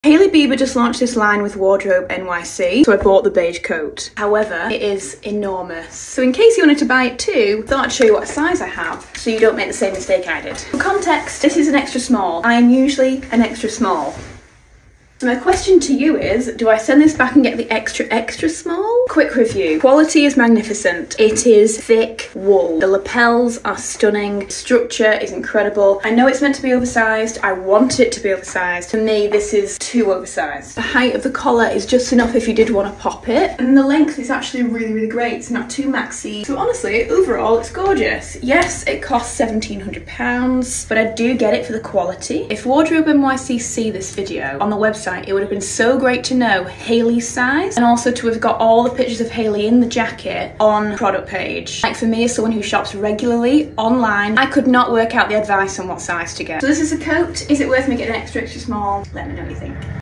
WATCH THIS BEFORE YOU BUY THE HAILEY BIEBER COAT
HAILEY BIEBERS COAT IS ENORMOUS - watch this before you buy

The hailey bieber Wardrobe NYC biscuit coat is the most beautiful thing I’ve seen for autumn but the size is massive so I wanted to show you my review so you don’t make the same sizing mistake I did :)